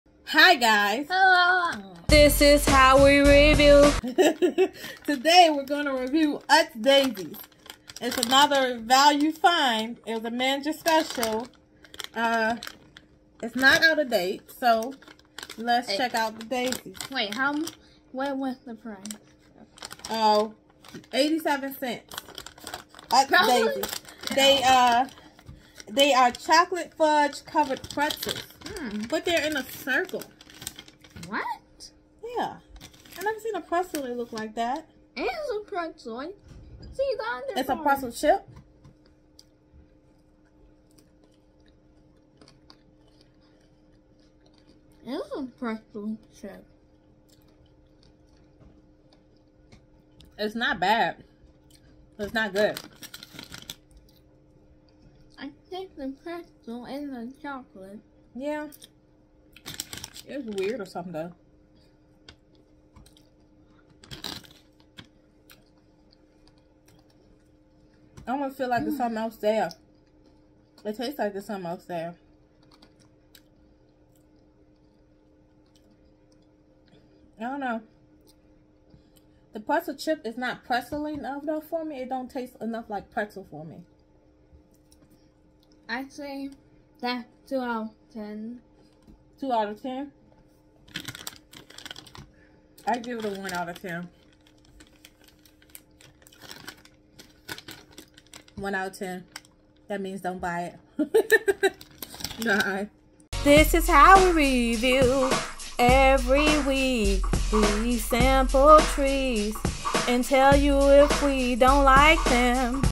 Chocolate Pretzel
We are reviewing another value find.